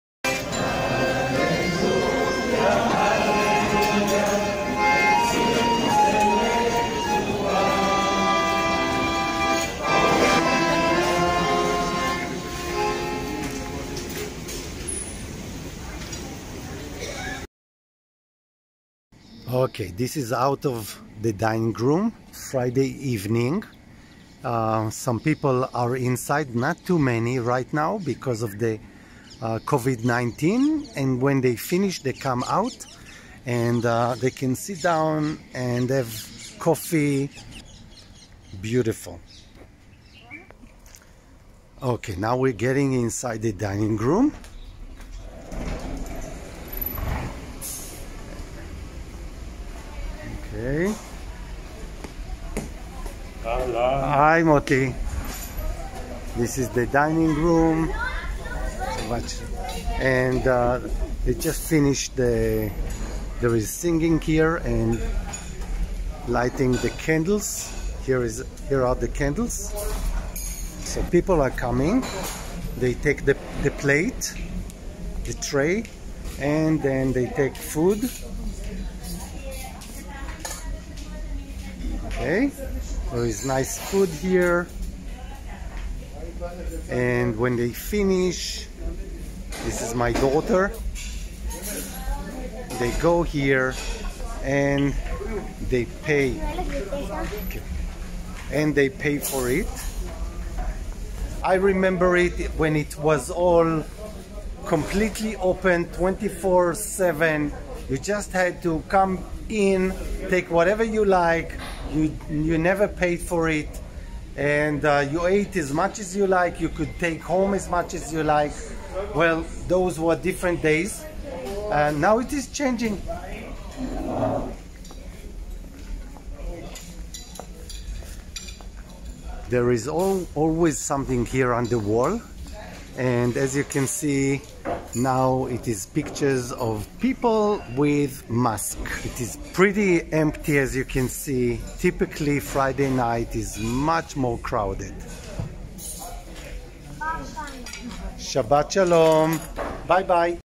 The DINING ROOM - My story
The DINING ROOM 🍜🍲🧆🍵🍅🥒 is the BEATING HEART 🧡 of the kibbutz.

For breakfast, lunch, and dinner, people are meeting in the dining room.

Since childhood, I remember Friday night Shabbat Dinner, when everybody dresses up with their Shabbat clothes, and the atmosphere is in some strange way 'holy', although we are not religious.